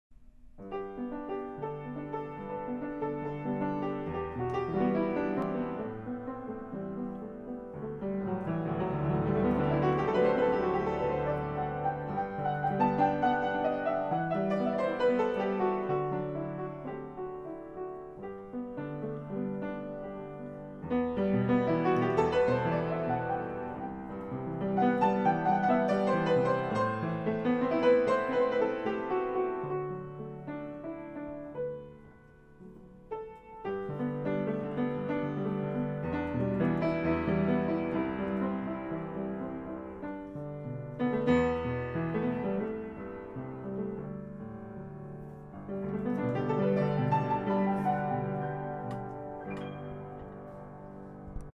Rare and beautiful Neubert German upright piano ( Klavier)
True   World war  1 and 2 survivor, play   great...  similar  sound  to  Bechstein, steinway,  bluthner   pianos.